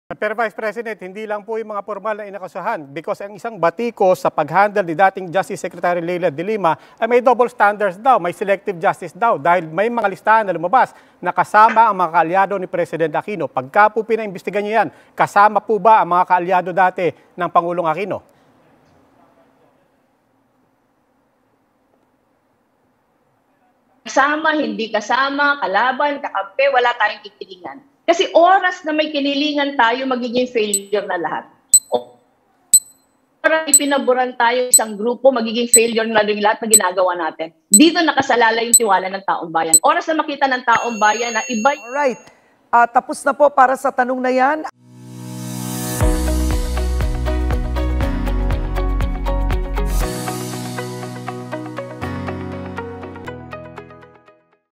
VP Robredo on going after Aquino allies if elected president
Presidential aspirant Vice President Leni Robredo highlights the importance of realigning the country's justice system with regards to holding people accountable for crimes against the nation, also stating that the Aquino allies will be taken into an investigation in opposition to the previously implemented selective justice proceedings.